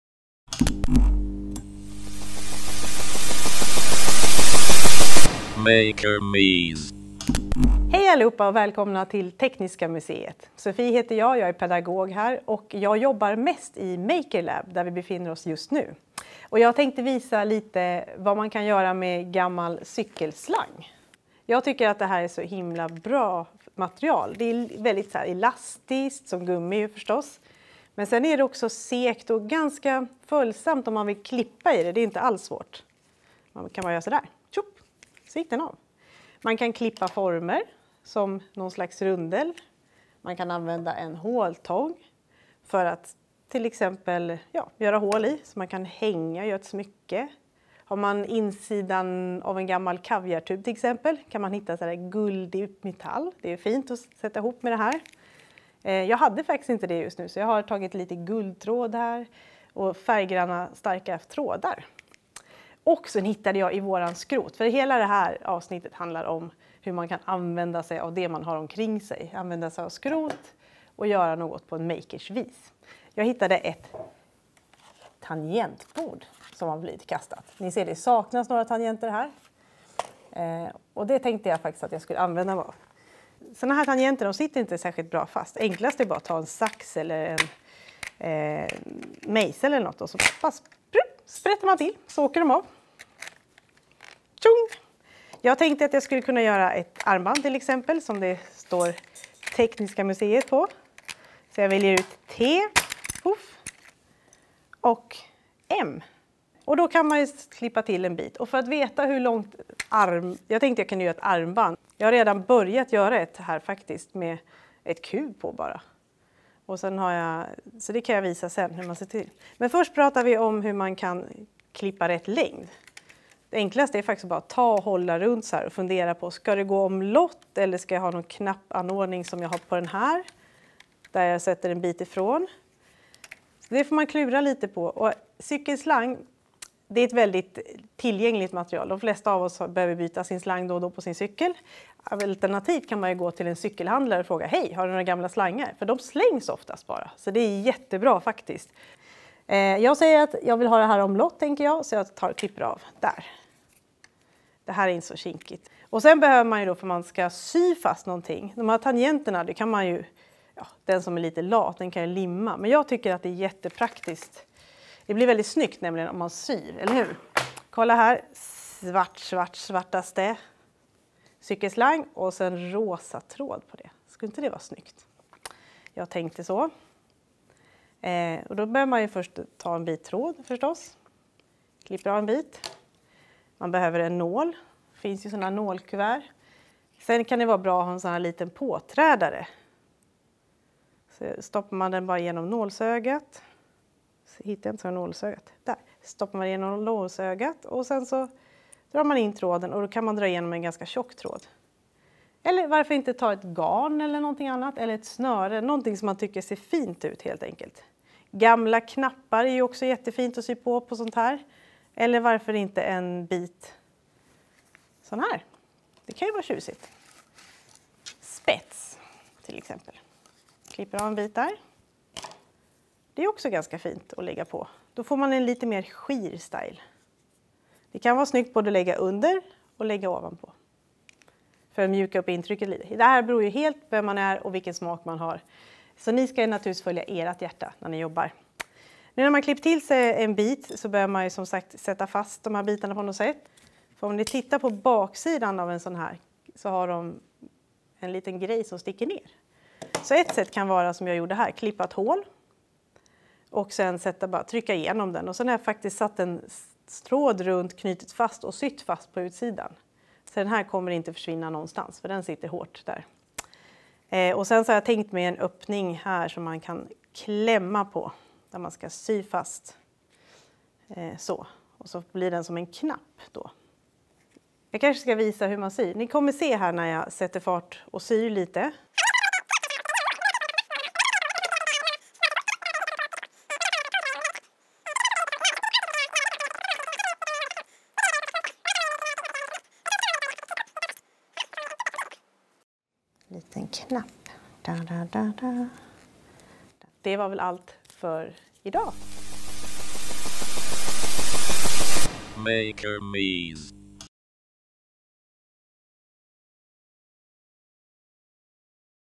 Makermys #2 - Gör ett armband av ett gammalt tangentbord
I dagens Makermys visar Sofie hur du kan göra ett eget armband av ett gammalt tangentbord och en cykelslang. Ett snyggt skrotsmycke är bättre än tio på tippen som vi brukar säga. Typ.

Makermys spelas in i Tekniska museets Makerlabb.